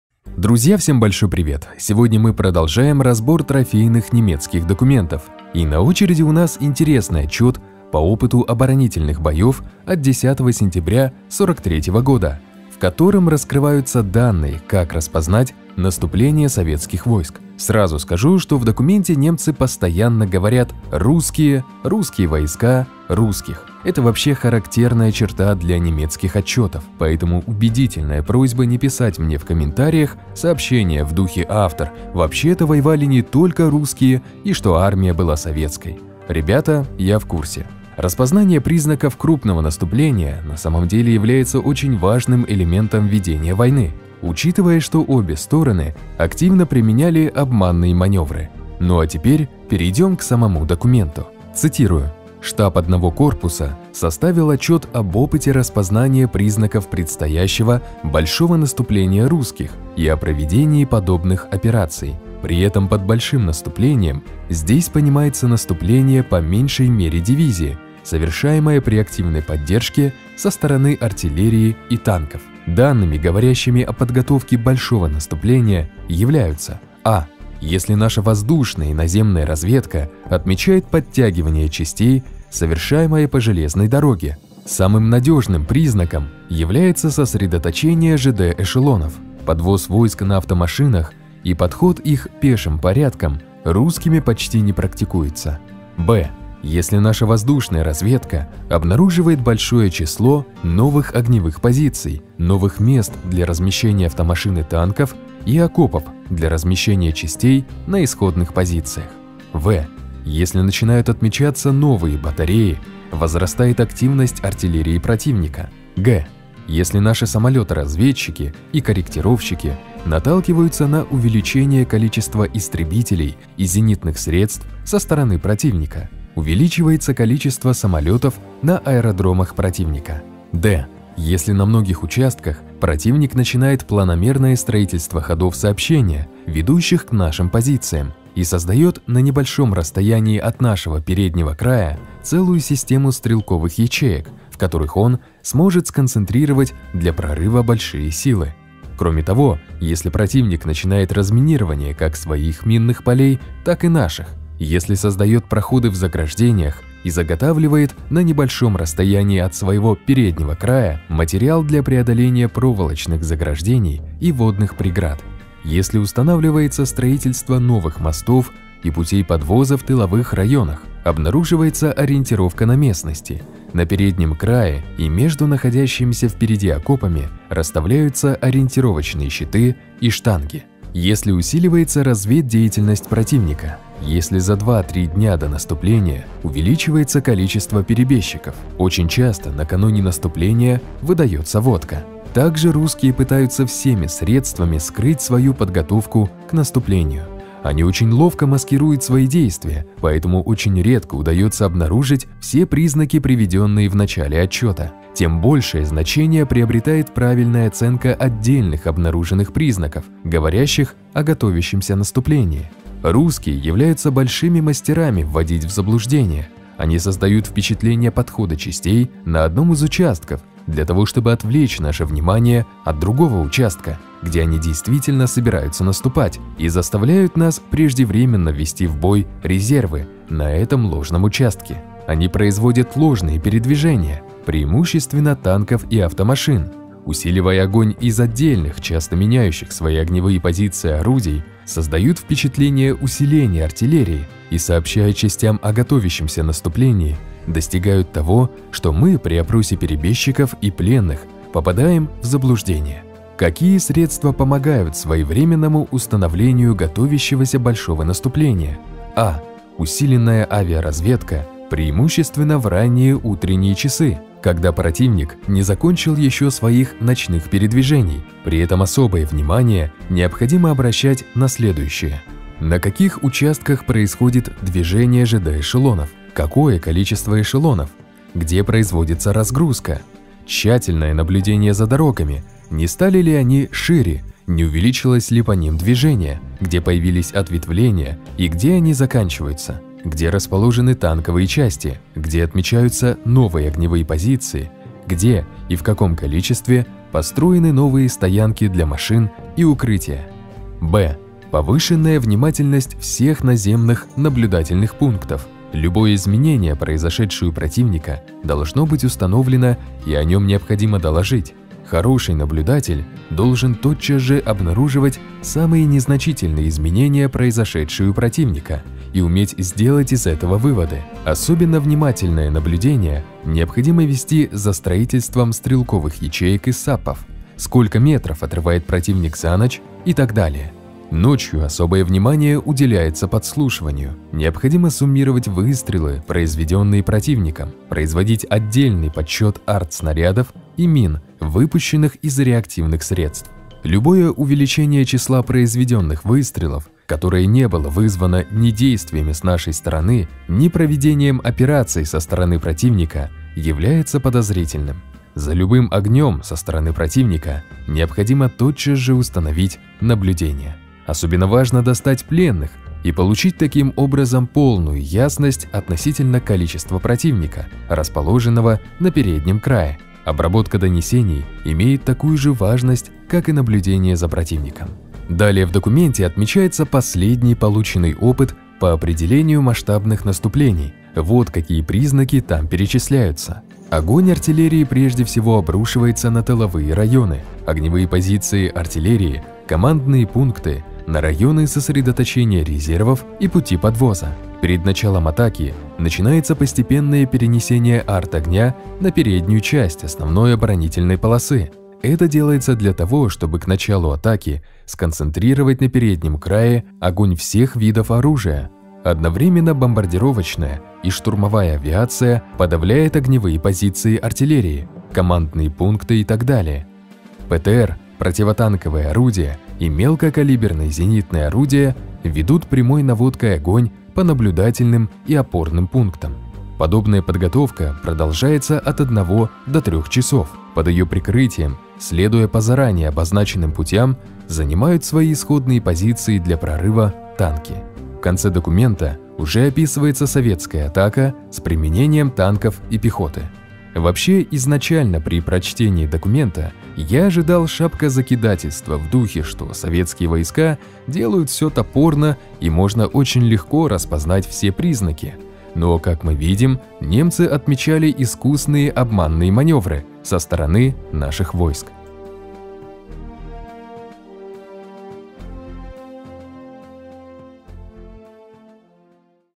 "Как Распознать Наступление Русских?" Немецкий Документ
Рассматриваем трофейный немецкий документ, в котором раскрываются признаки наступления Советских Войск